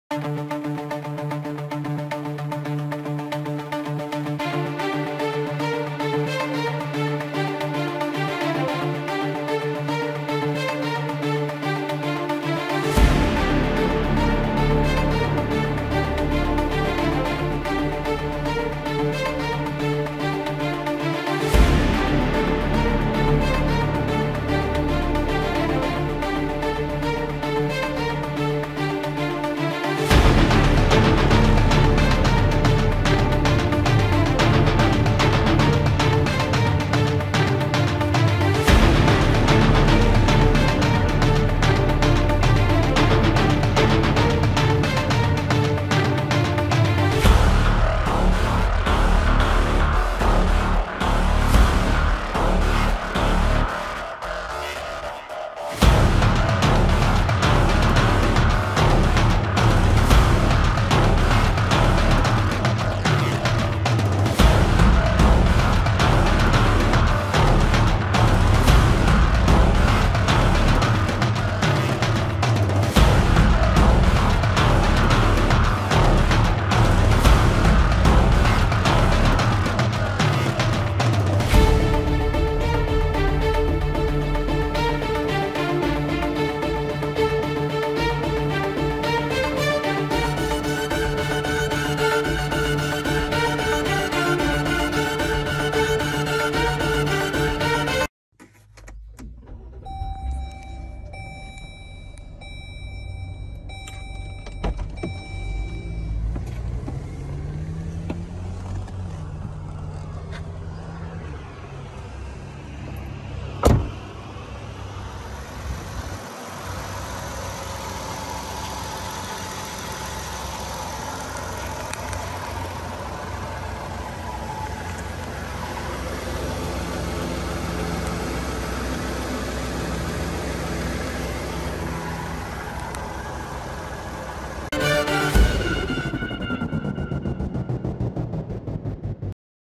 For Sale 2018 Chrysler Pacifica ATS Vans Manual Fold Ramp Rear Loading Wheelchair Van
Sale priced $34,995 with Free Nationwide Shipping For Sale 2018 Chrysler Pacifica ATS Vans Manual Fold Ramp Rear Loading Wheelchair Van